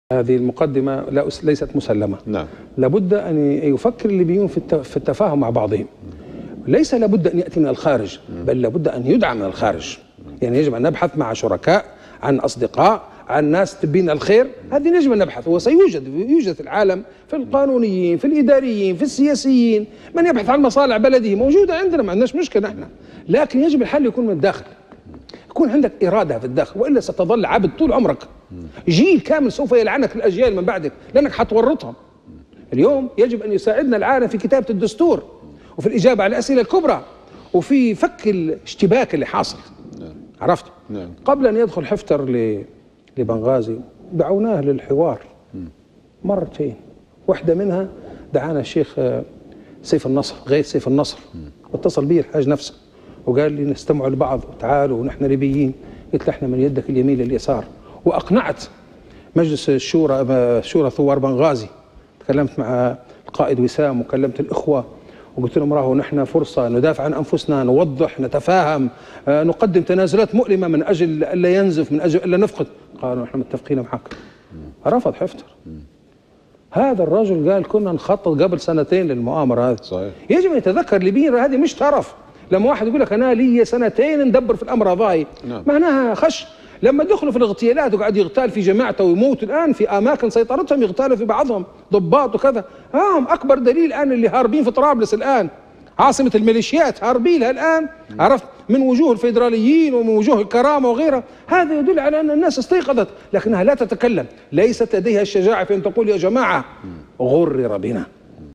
من حوارات التناصح | السيد محمد عمر بعيو | الكاتب والباحث السياسي |حفتر لايريد سلاماً
من حوارات التناصح | السيد محمد عمر بعيو | الكاتب والباحث السياسي |حفتر لايريد سلاماً | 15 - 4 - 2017

ــــــــــــــــــــــــــــــــــــــــــــــــــــــــــــــــــــــــــ
ــــــــــــــــــــــــــــــــــــــــــــــــــــــــــــــــــــــــــ
يمكنكم تحميل تطبيقنا TANASUH TV على الهواتف الذكية: